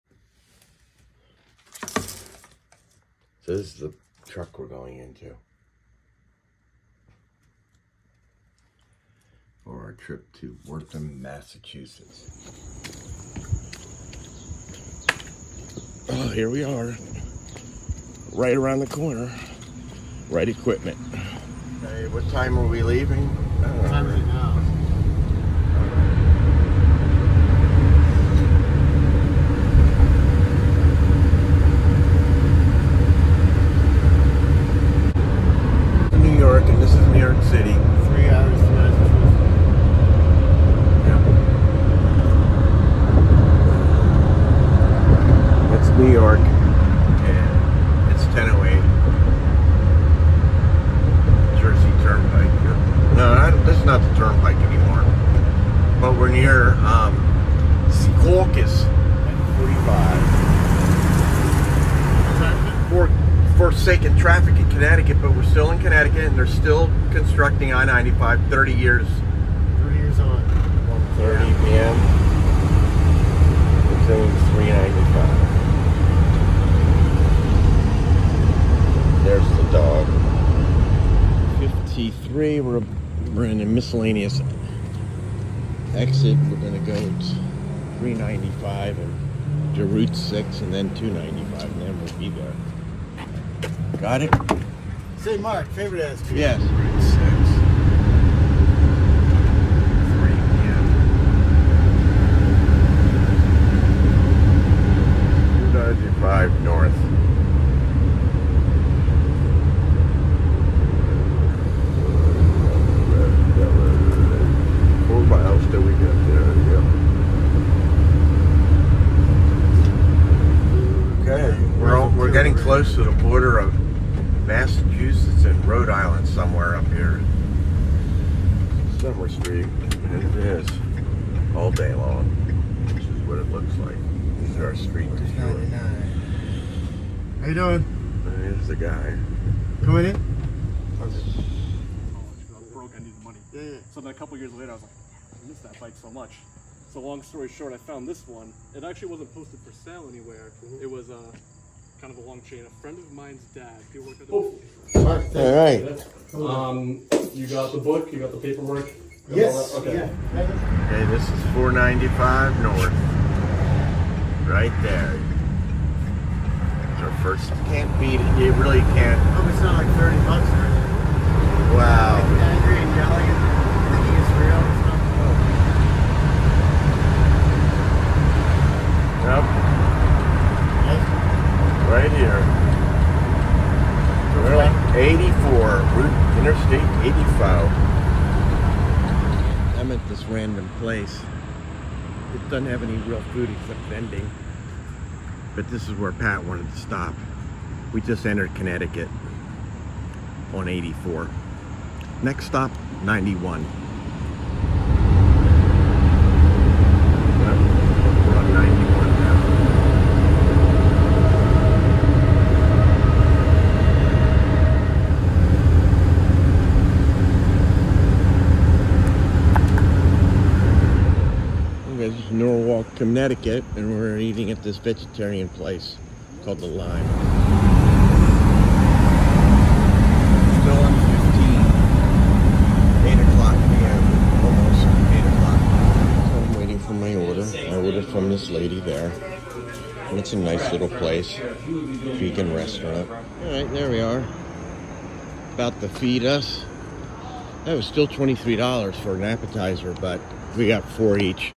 Saturday, August 26, 2023
We went to a road trip to Wrentham messages to pick up a motorcycle pack purchased. He’s a very nice day for a road trip. We arrive there around 4 PM. Grabbed a bite to eat at 8 PM. Got home at 11:30 PM.